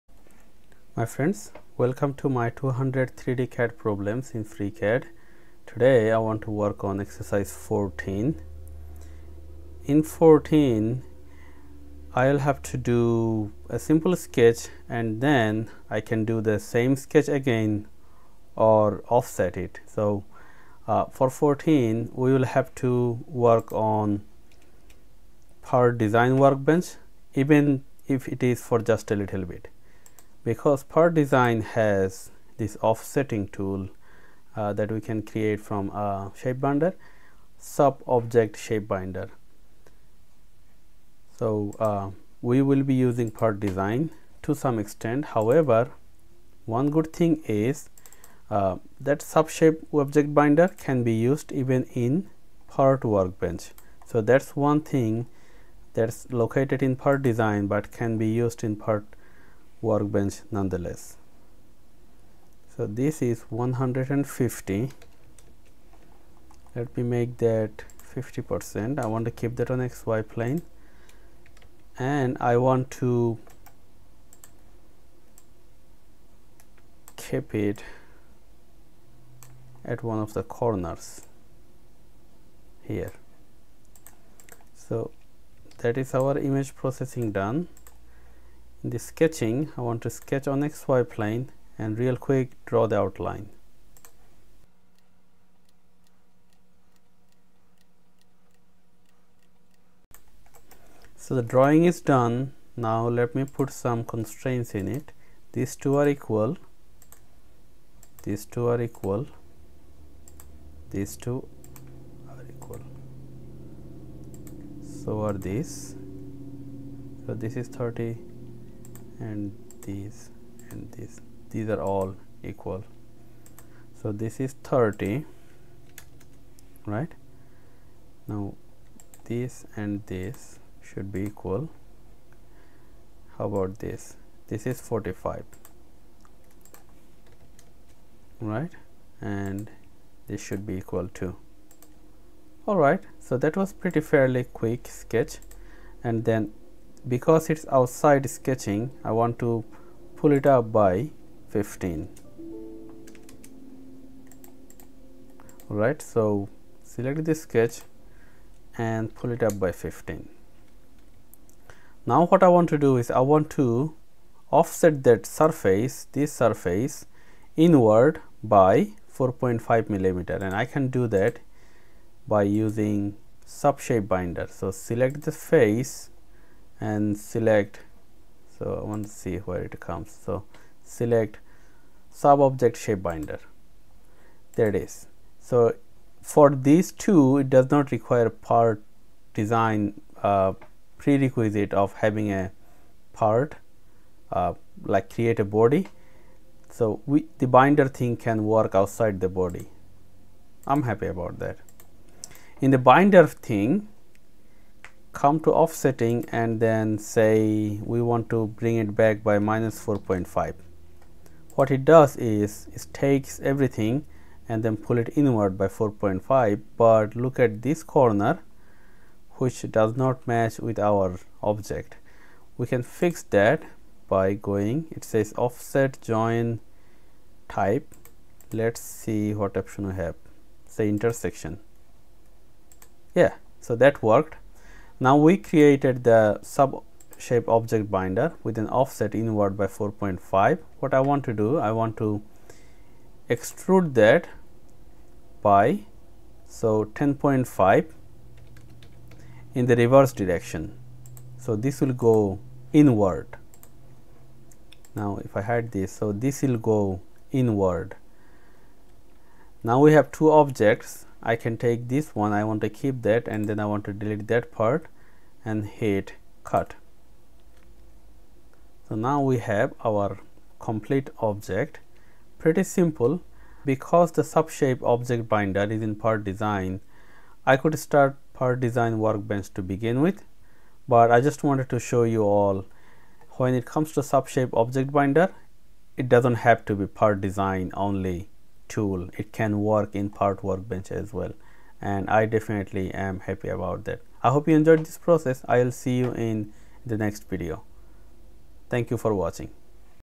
200 3D Models in FreeCAD: Exercise 14 (Offset a Sub-Object Shape Binder)
Update: I could have used the "2D Offset" tool from Part workbench.
What if we blended Part and Part Design in one single workbench and name it, say, "Basic Tools".